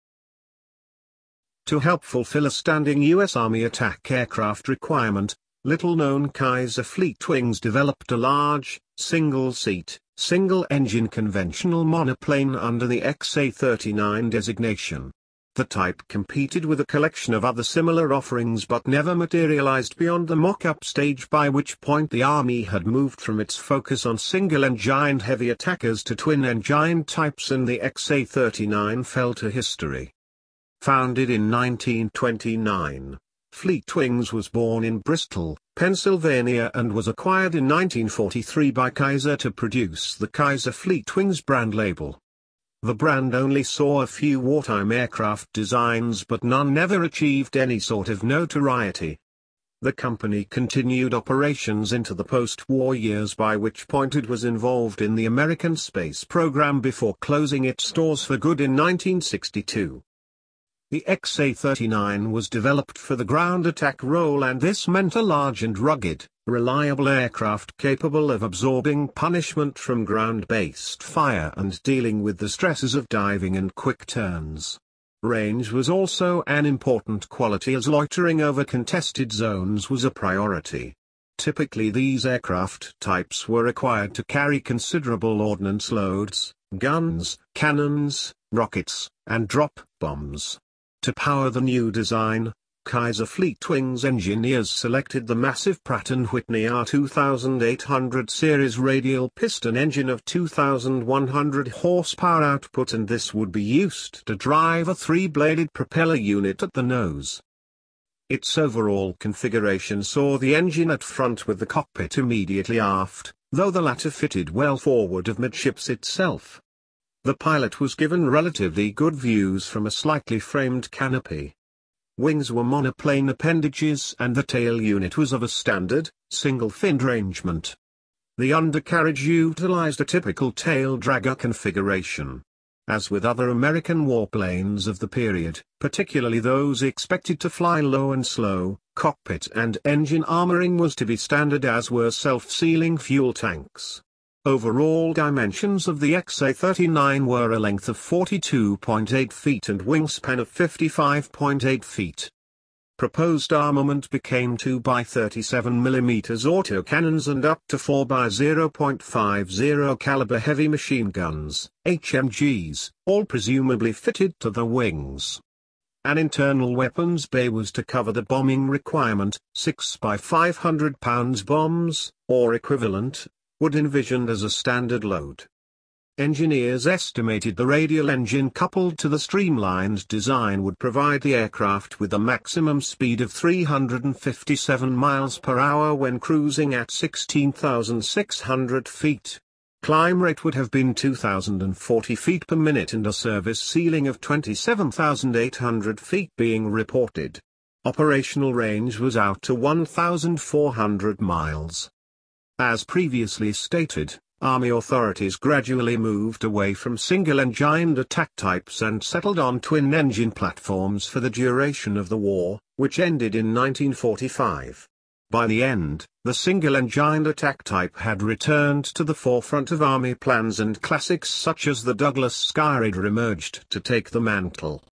Kaiser-Fleetwings XA-39 Ground Attack Aircraft Prototype
Details of the proposed Kaiser-Fleetwings XA-39 attack aircraft of the World War 2 period.